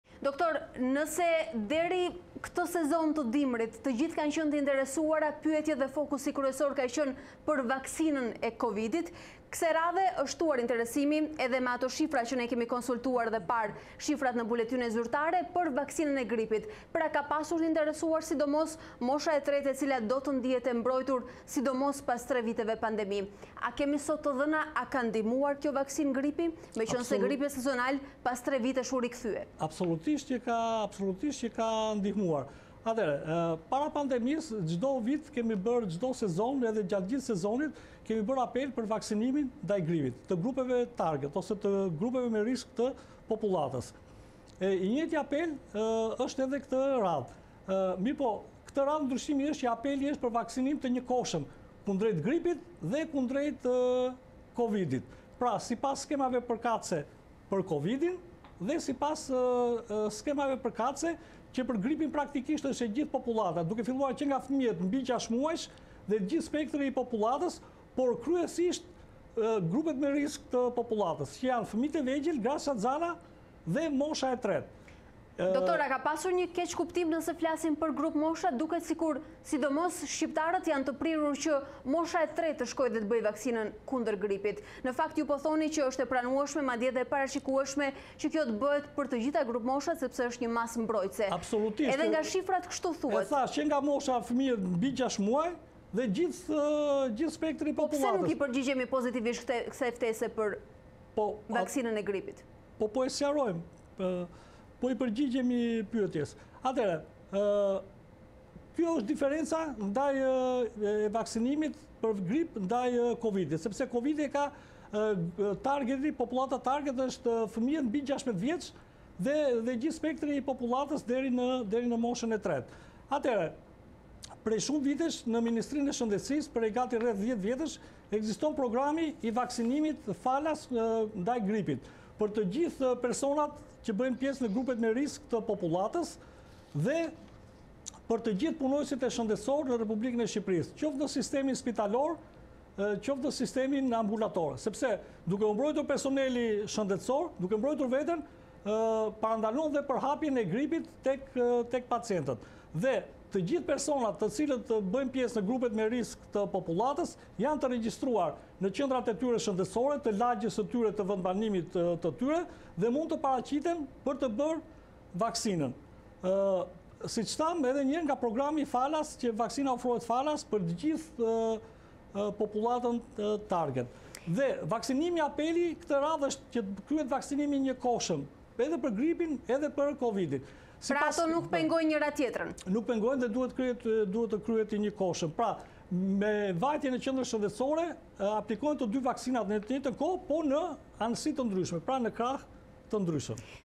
Gripi dhe pandemia bashkëjetojnë, mjeku: Piku në fund të janarit, të kryhet vaksinimi i njëkohshëm
Rastet e gripit në vend janë shtuar, dhe sipas mjekëve piku do të jetë në fund të janarit, si edhe në muajin shkurt. Sipas Artan Simakut, nga Instituti i Shëndetit Publik, situata gjithsesi paraqet një trend të pritshëm, edhe pse agresiv. Simptomat e gripit stinor janë shumë të ngjashme me agresivitetin e COVID-19, aq sa ka shpesh herë konfuzion mes të dyjave, por Simaku jo vetëm pohon se ekziston tashmë një sistem testimi për të dy viruset, por bën një apel për vaksinimin  e popullatës.